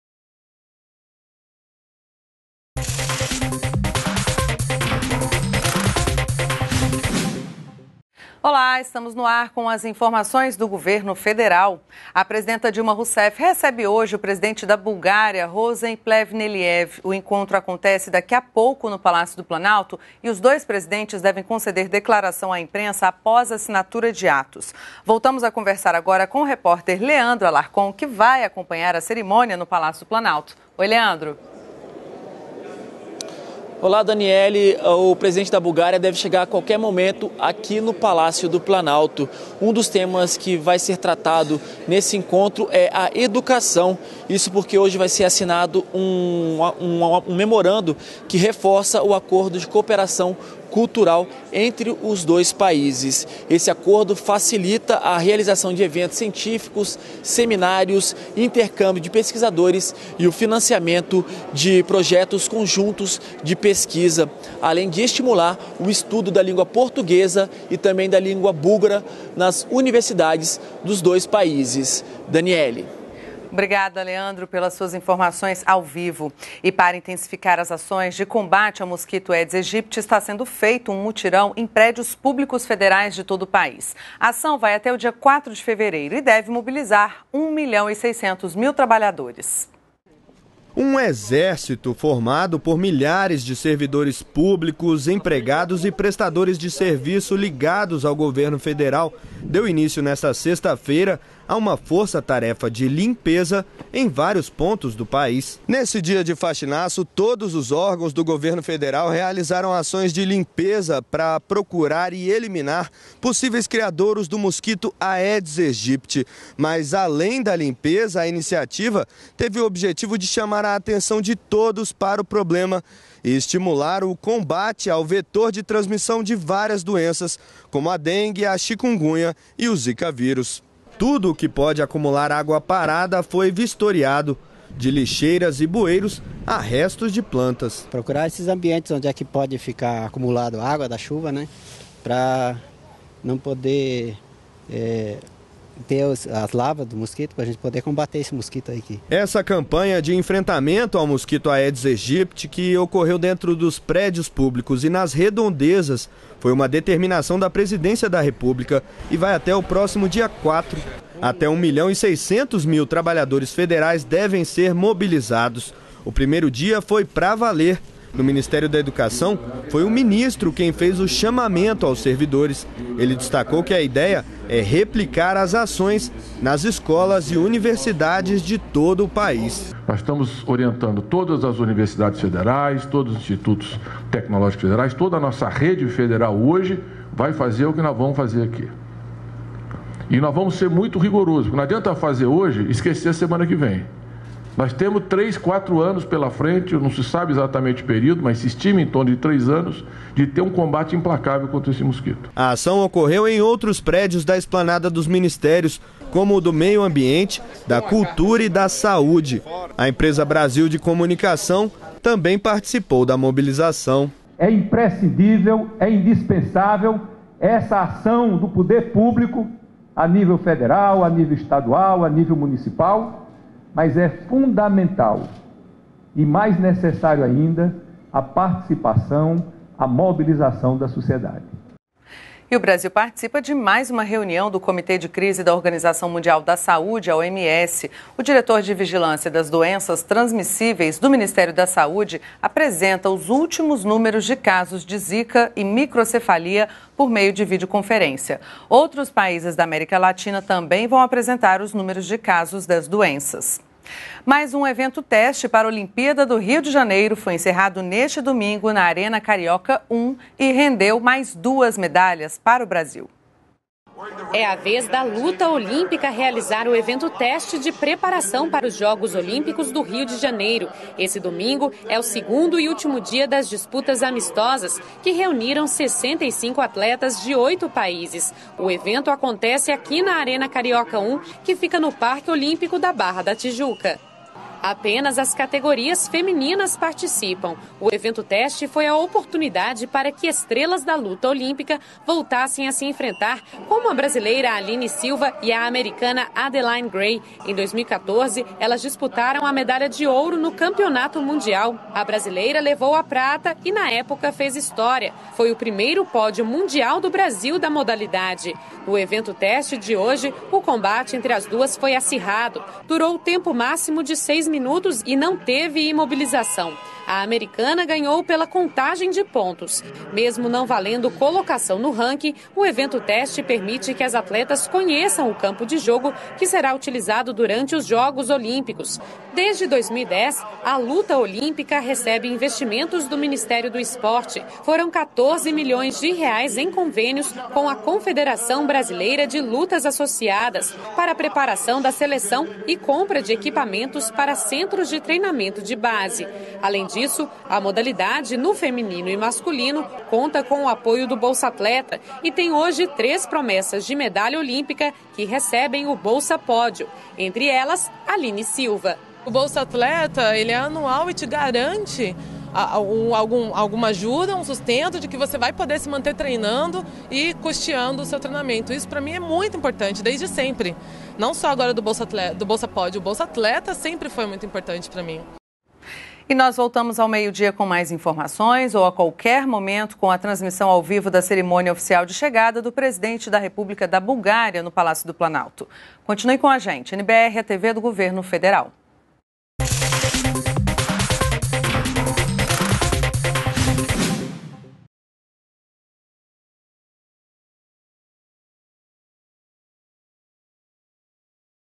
Últimas - Brasil e Bulgária devem fechar acordo para facilitar intercâmbio de pesquisadores
ÚLTIMAS 11h - 01.02.16: A presidenta Dilma Rousseff recebe nesta segunda-feira (1º) a visita oficial do presidente da Bulgária, Rosen Plevneliev. Os chefes de Estados devem assinar diversos acordos de cooperação, como o que facilita a realização de eventos e o intercâmbio de pesquisadores da área de educação. Cerca de 1,6 milhão de servidores públicos participam de mutirão contra o Aedes Aegypti até o próximo dia 4 de fevereiro. Brasil participa de nova reunião mundial sobre o vírus Zika. E mais: Rio de Janeiro sedia evento-teste da Luta Olímpica.